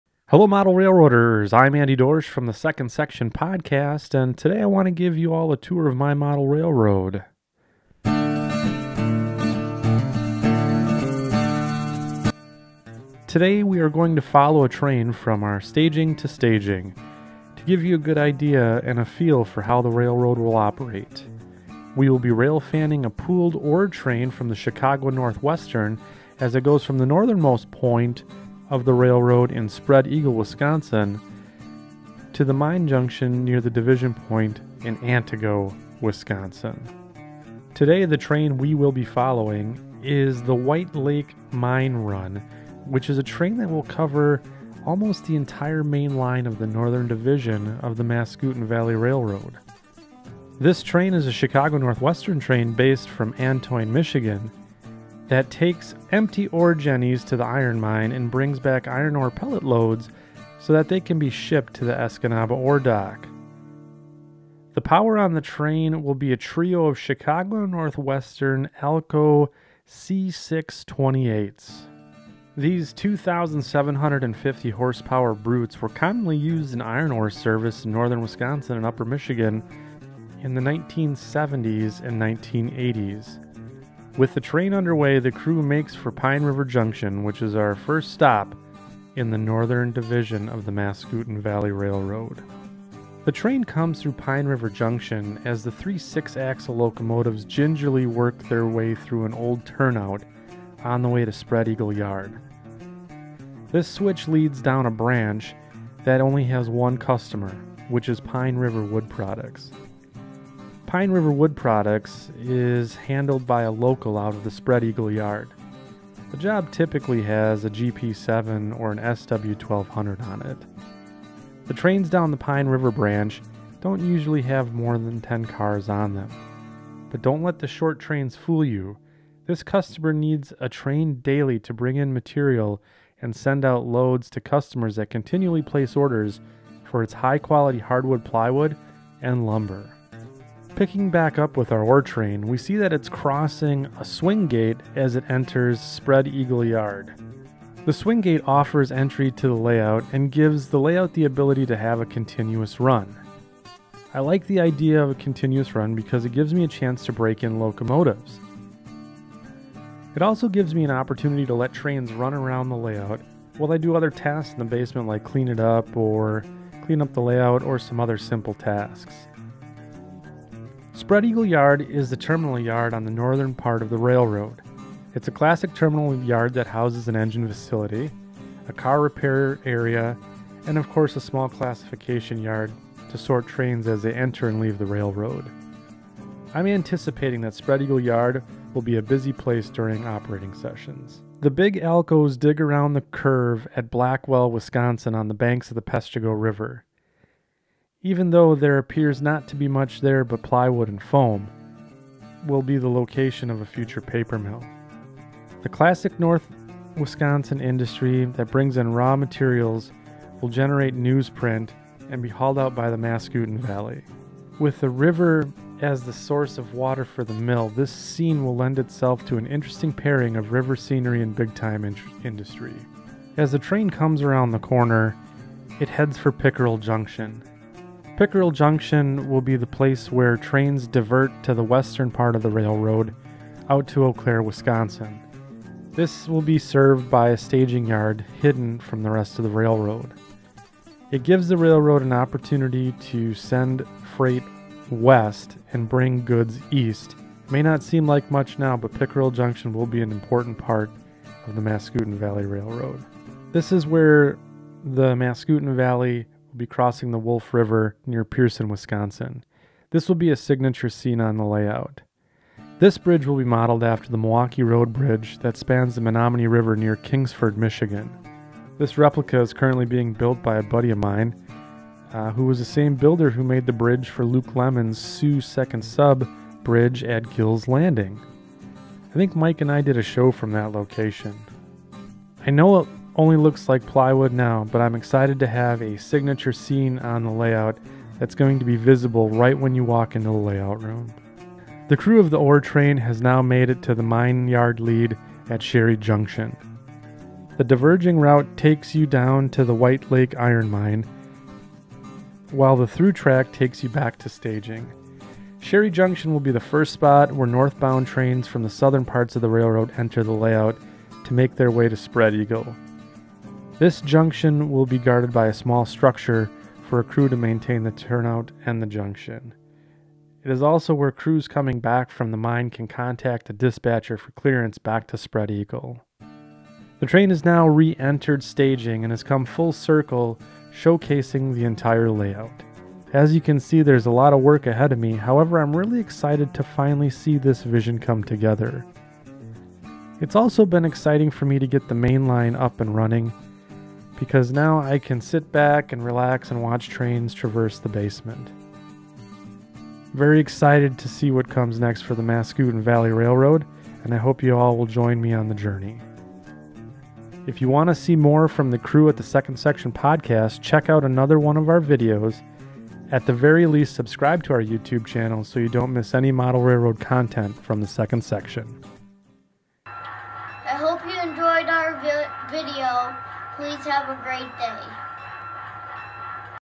Model Railroad Layout Tour Mascoutin Valley Railroad
Hello Model Railroaders! In this video Andy gives you a tour of the layout as we follow a Southbound Ore Empty back all away around the layout. Join him as he sends a train around the HO Scale Mascoutin Valley Railroad!

Wanna be part of the Section Crew?